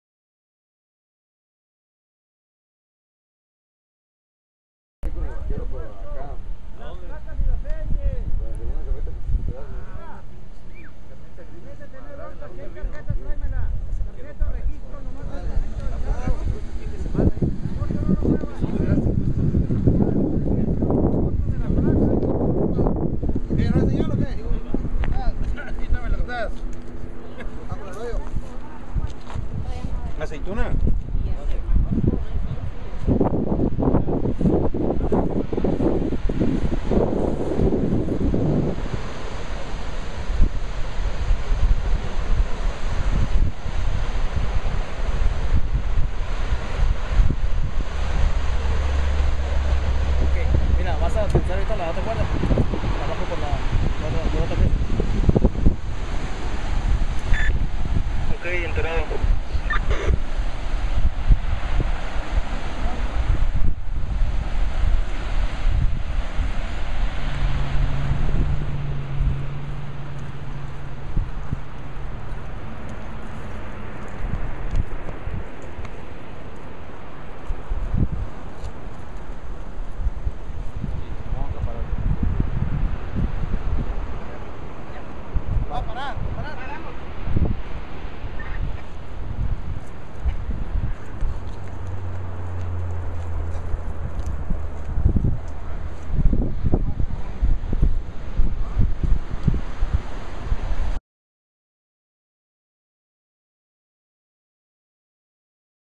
Suicidio en la Escénica
Un hombre se lanzó por un acantilado en las inmediaciones de Salsipuedes dentro de su auto. Dejó una nota póstuma.